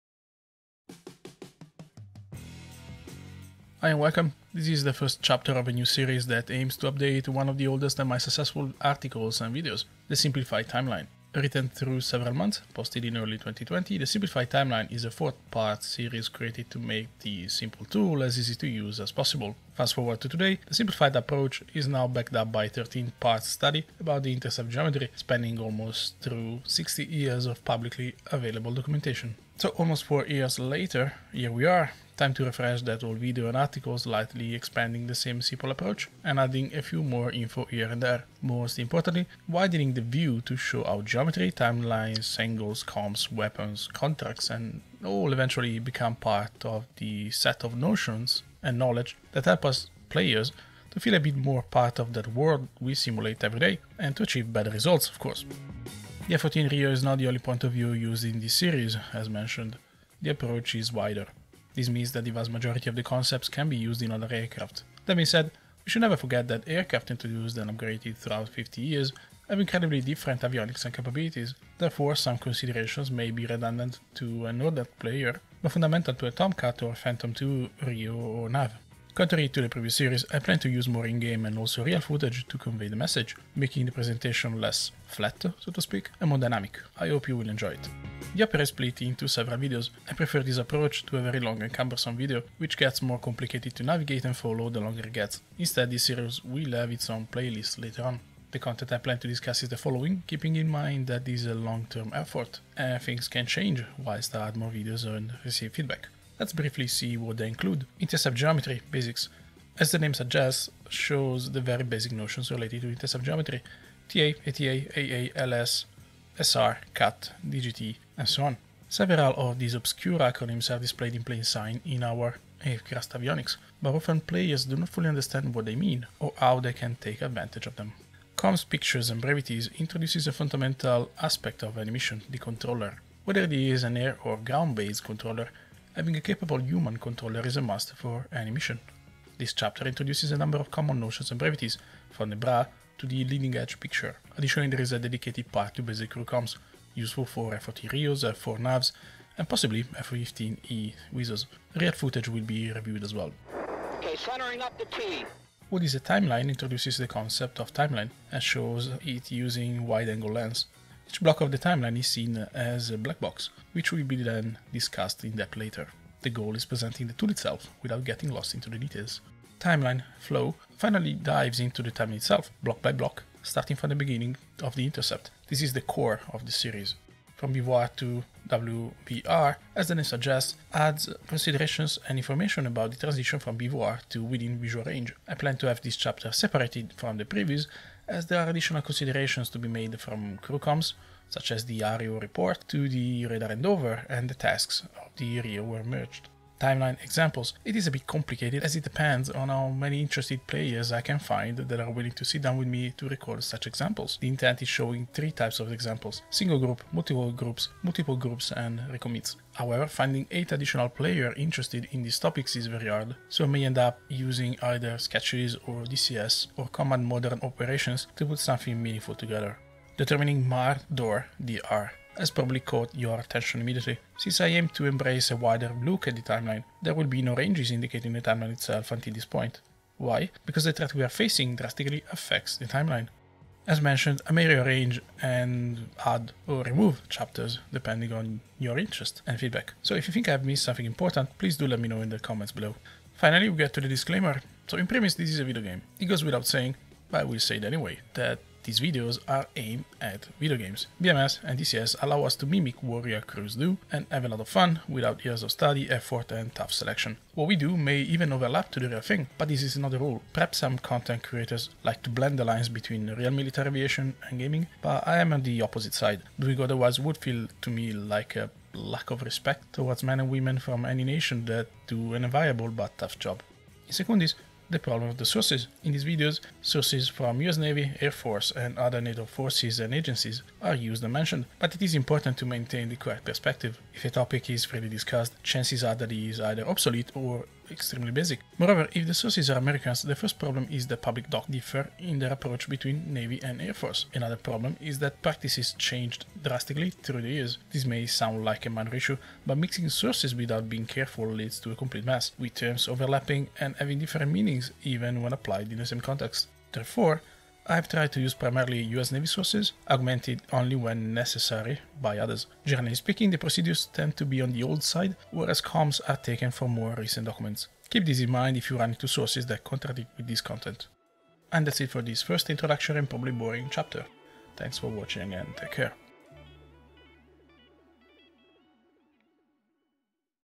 Introduction and Disclaimer - BVR Timeline - Chapter 1
This introductory part of the new BVR Timeline series presents the chapters that compose it and clarifies the sources used.

This series is an update and upgrade of the 4-parts discussion about the Simplified Timeline I published in early 2020.
The goal is maintaining the same simple approach whilst expanding the study to make it more flexible and thus suitable to multiple aircraft, but without going fully into the details of the several topics covered.

0:00 - Introduction
0:55 - Perspective
1:11 - Table of contents
1:47 - Contents in detail
4:03 - Disclaimer